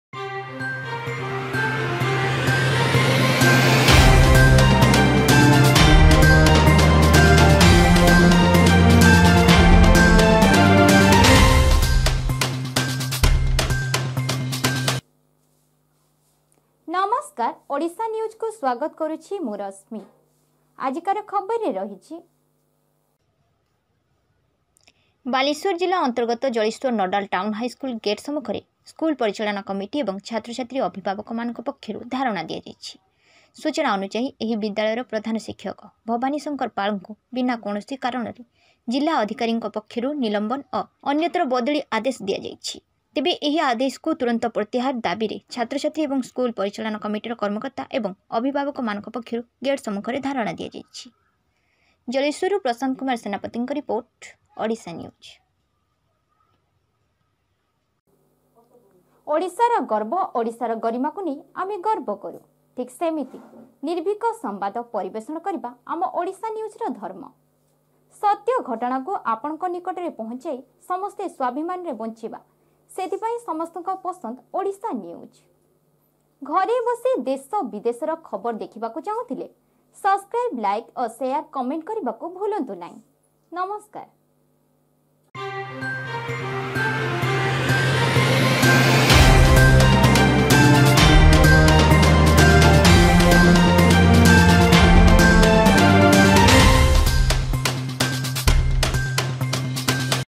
ଜଳେଶ୍ବର ନୋଡାଲ ଟାଉନ ହାଇସ୍କୁଲ ଗେଟ ସମ୍ମୁଖରେ ଛାତ୍ର ଛାତ୍ରୀ ଅଭିଭାବକ ମାନକଂ ପକ୍ଷରୁ ଧାରଣା ଦିଆଯାଇଛି
ବାଲେଶ୍ବର ଜିଲ୍ଲା ଅନ୍ତର୍ଗତ ଜଳେଶ୍ବର ନୋଡାଲ ଟାଉନ ହାଇସ୍କୁଲ ଗେଟ ସମ୍ମୁଖରେ ସ୍କୁଲ ପରିଚାଳନା କମିଟି ଏବଂ ଛାତ୍ର ଛାତ୍ରୀ ଅଭିଭାବକ ମାନକଂ  ପକ୍ଷରୁ ଧାରଣା ଦିଆଯାଇଛି